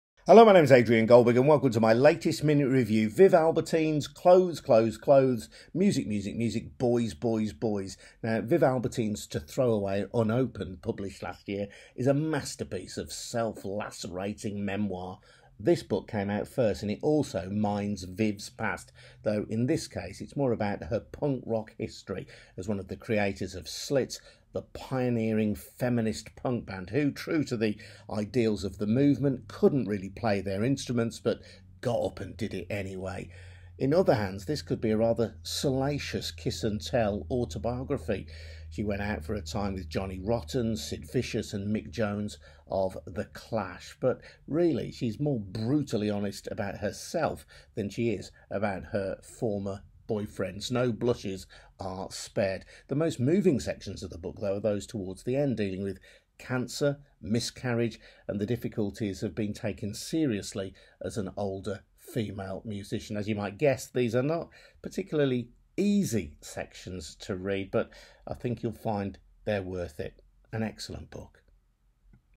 CLOTHES, CLOTHES, CLOTHES BY VIV ALBERTINE (Adrian Goldber's Minute Review)
Adrian Goldberg's Minute Review of Clothes, Clothes, Clothes, Music, Music, Music, Boys, Boys, Boys by Viv Albertine